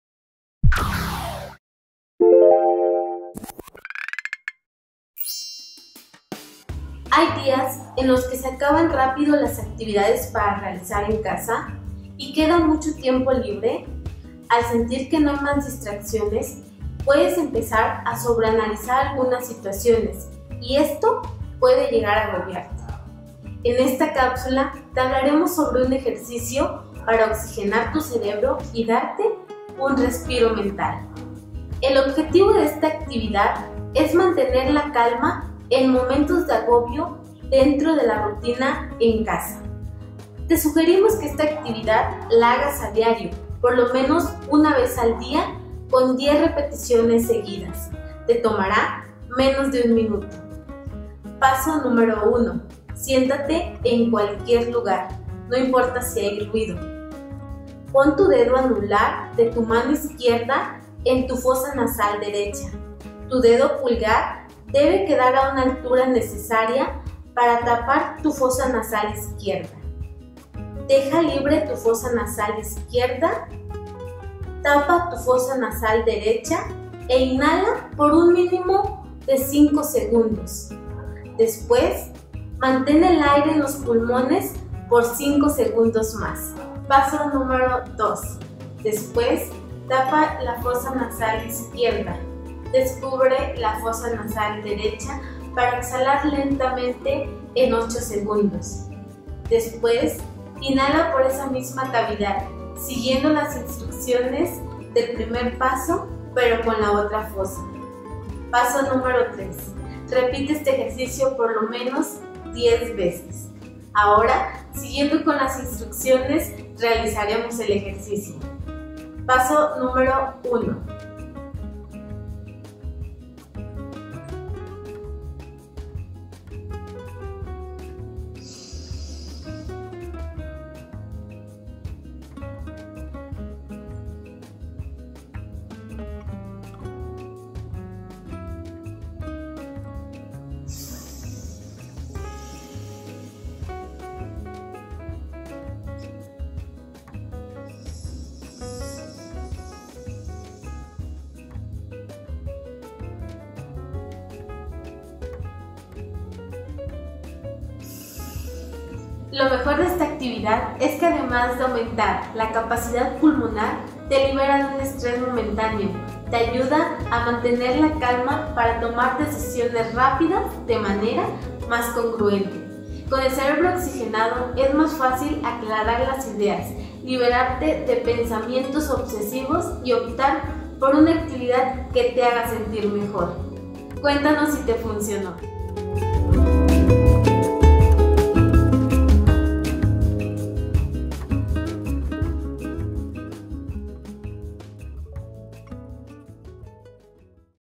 Date un respiro mental.
Cápsula con Cecilia Hernández del área de atención al público del museo.